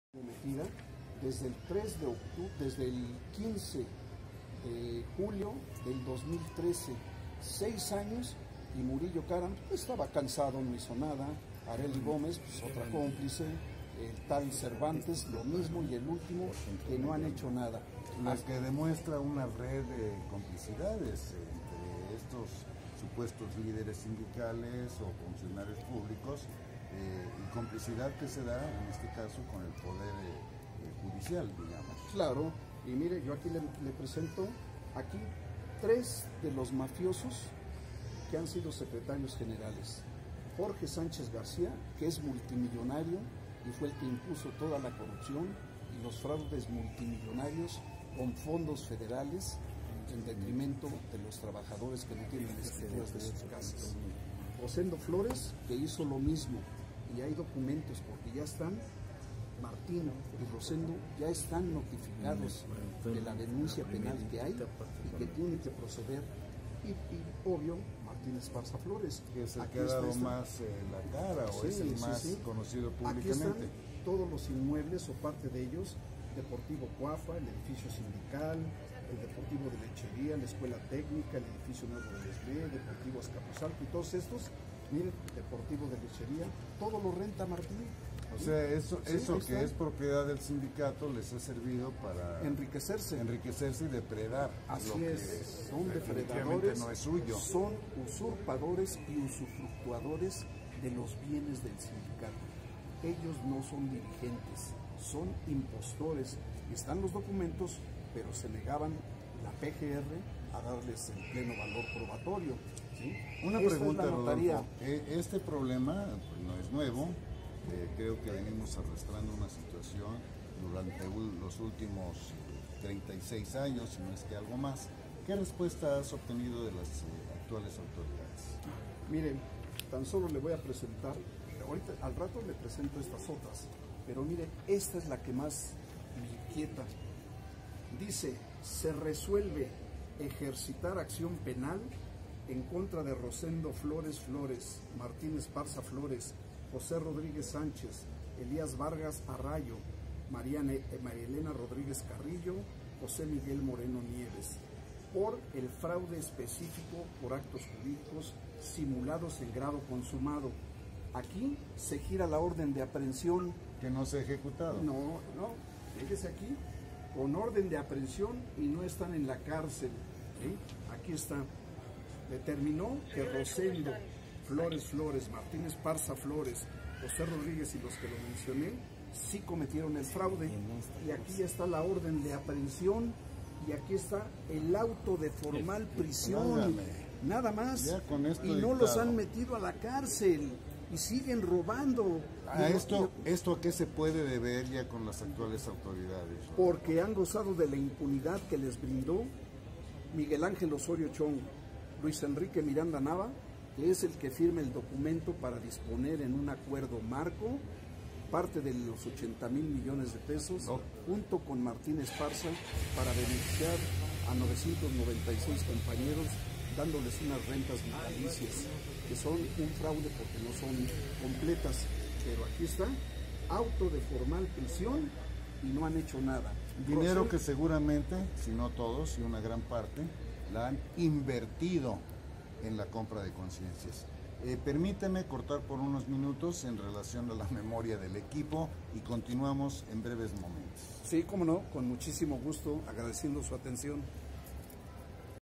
ENTREVISTA A RODOLFO BONILLA 3-5
Entrevista de Miguel Angel Ortiz a Rodolfo Bonilla, asesor jurídico de los trabajadores del Sindicato Mexicano de Electricistas (SME).